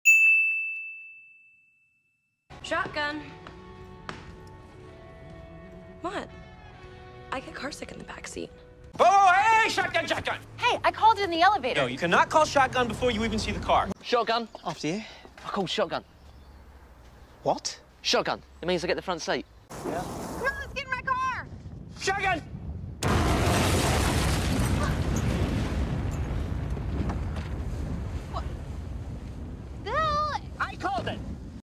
English slang “shotgun” 🚗
“Shotgun” is said when someone wants to claim the right to sit in the front seat.

The act of claiming shotgun is called “calling shotgun”.

The act of sitting in the front passenger seat is called “riding shotgun”.